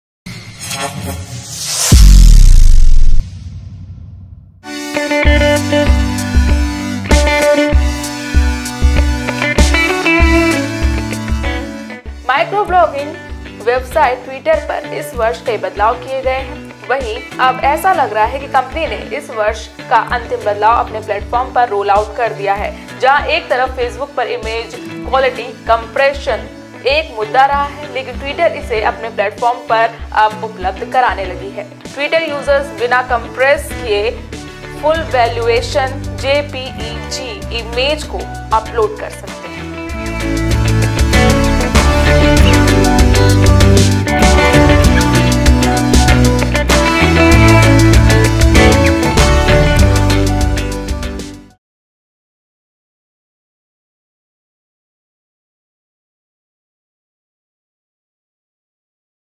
Facebook को अब मात देगा twitter - HM NEWS
माइक्रोब्लॉगिंग वेबसाइट Twitter पर इस वर्ष कई बदलाव किए गए हैं। वहीं, अब ऐसा लग रहा है कि कंपनी ने, इस वर्ष का अंतिम बदलाव अपने प्लेटफॉर्म पर रोलआउट कर दिया है जहां एक तरफ Facebook पर इमेज क्वालिटी कंप्रेशन ,एक मुद्दा रहा है